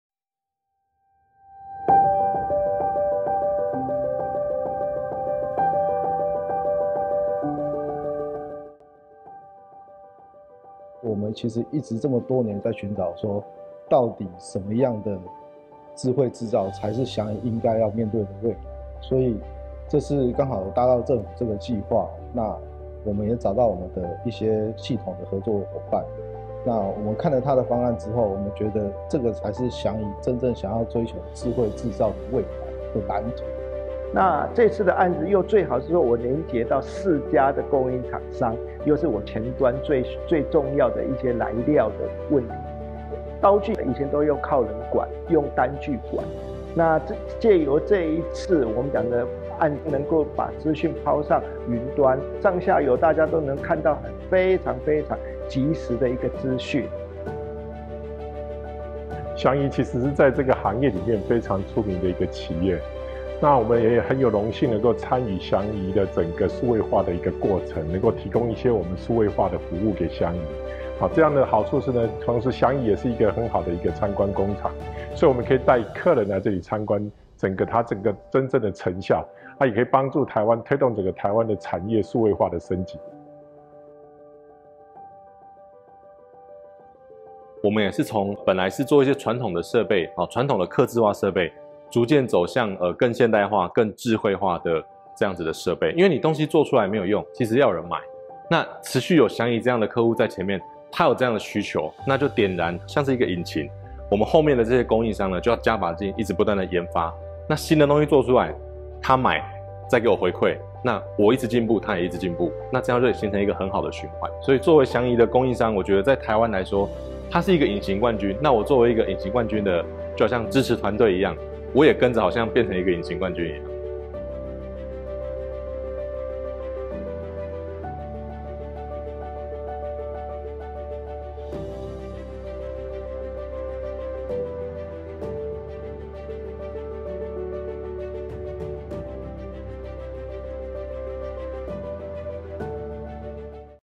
【帶領上下游一起數位升級，祥儀企業能耗監測、智能化工廠、供應鏈串流一次到位】
近年來機械產業景氣變化快速，考驗加工的準確性及彈性應變力，祥儀「以大帶小」進行智慧製造零件加工數位轉型供應鏈計畫，以智慧化生產為目標，提升生產效率，同時監測能耗，朝低碳轉型的方向前進。